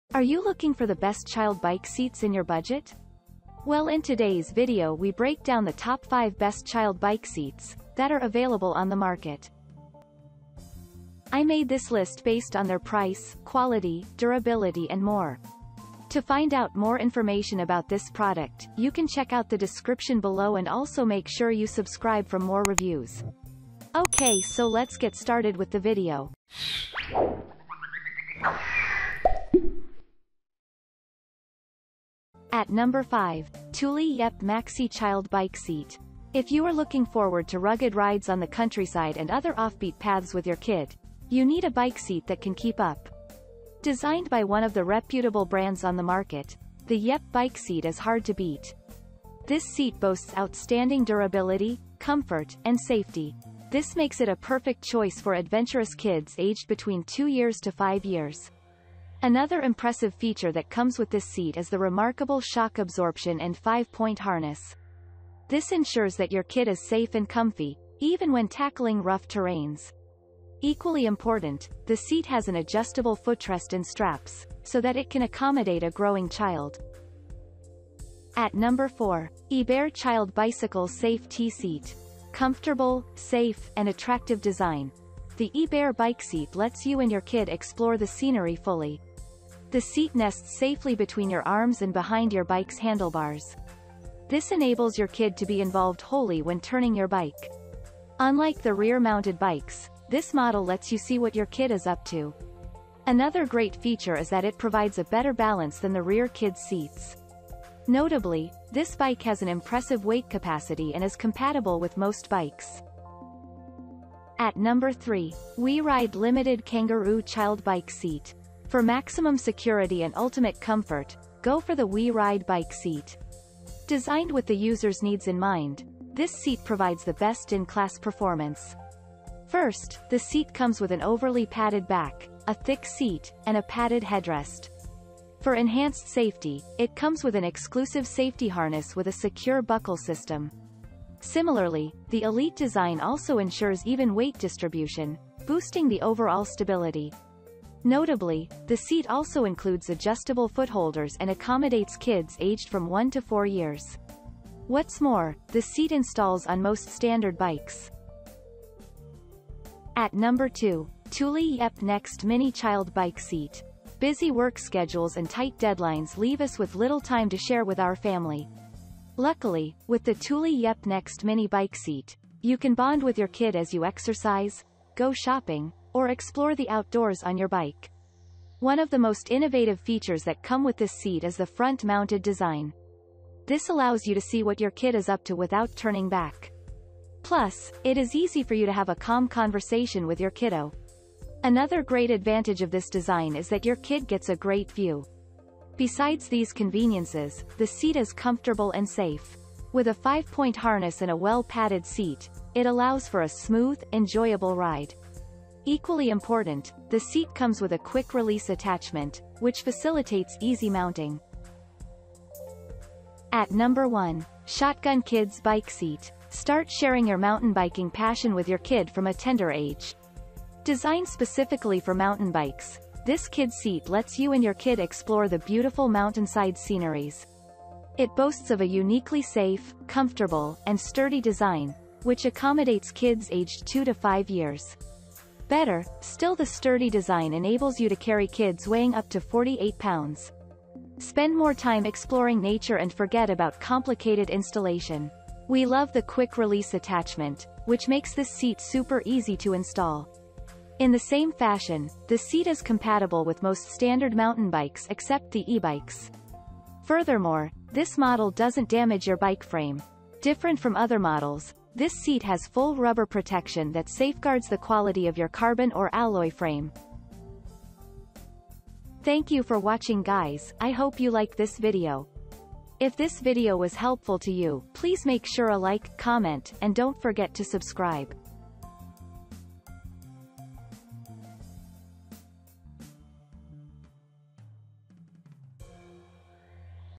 Top 5 Best Child Bike Seats in 2022 Reviews
☛ All The Links to Child Bike Seats Listed in this Video:-

▶️ 5. Thule Yepp Maxi Child Bike Seat.

▶️ 4. iBert Child Bicycle Safe-T-Seat.

▶️ 3. WeeRide LTD Kangaroo Child Bike Seat.

▶️ 2. Thule Yepp Nexxt Mini Child Bike Seat.

▶️ 1. SHOTGUN Kids Bike Seat.